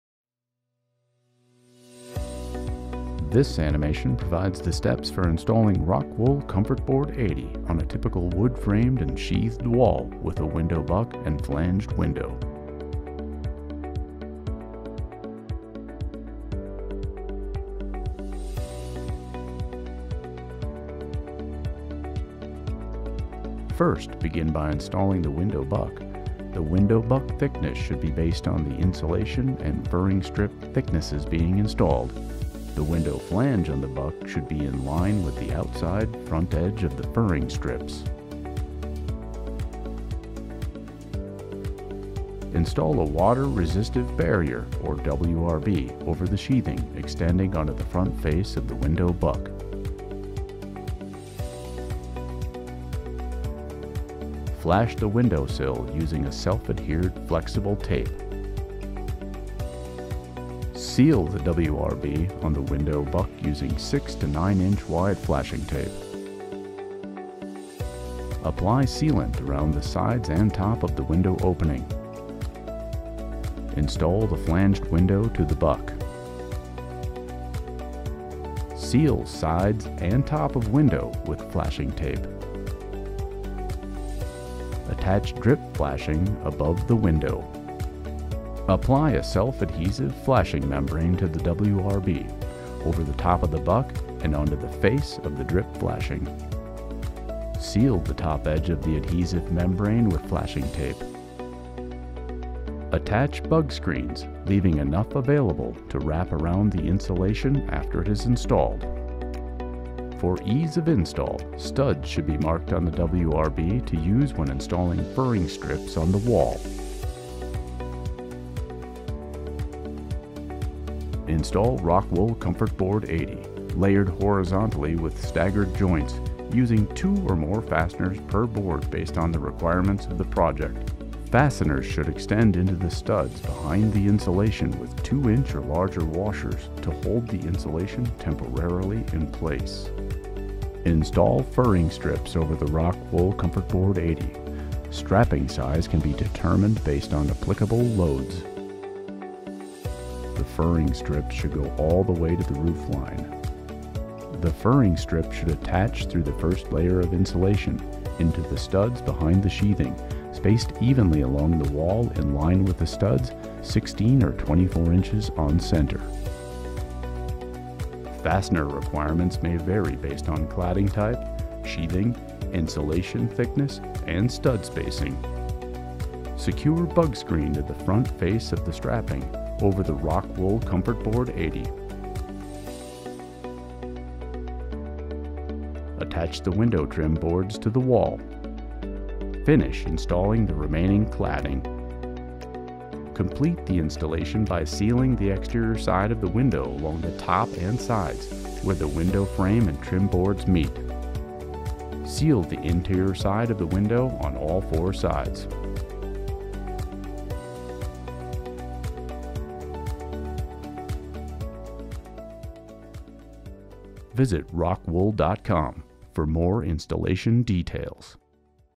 How To: ROCKWOOL Comfortboard® 80 Around A Window Buck Installation
ROCKWOOL Comfortboard 80 is a rigid stone wool insulation board designed for exterior continuous insulation assemblies using screw through insulation cladding support systems in residential and commercial construction.

Learn how Comfortboard 80 is installed around a non-flanged window with a window buck on a residential, wood-framed home. This installation includes guidance on insulation fastening, cutting and detailing around a window buck, as well as recommendations on air and water barrier continuity.
Please be sure to follow the window, water-resistive barrier and cladding manufactures instructions during installation.

Have more questions? Contact the ROCKWOOL technical support team